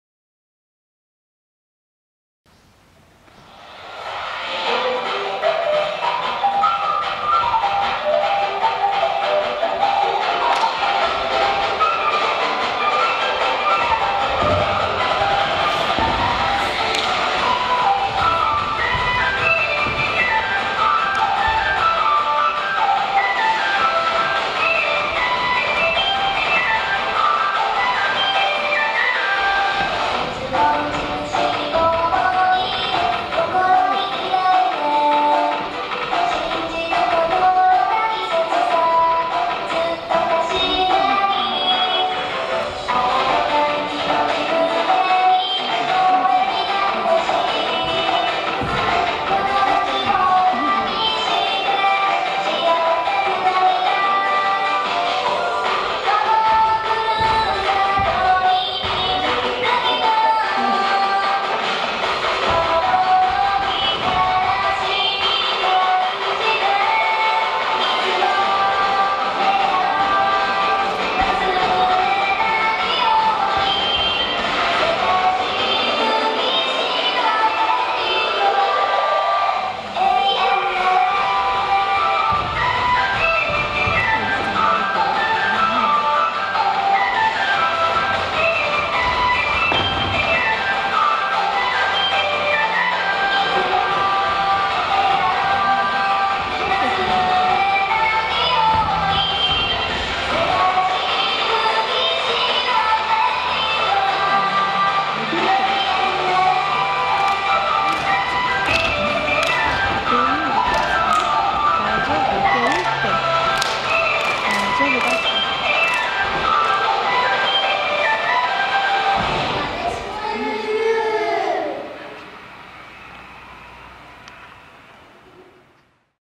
「1000人クッキーダンス踊ってみた」～江面第一小学校なでしこゆうゆう編～
江面第一小学校なでしこゆうゆうのみなさんが、クッキーダンスを踊ってくれました！元気いっぱいのダンスは見ていて楽しくなってきます♪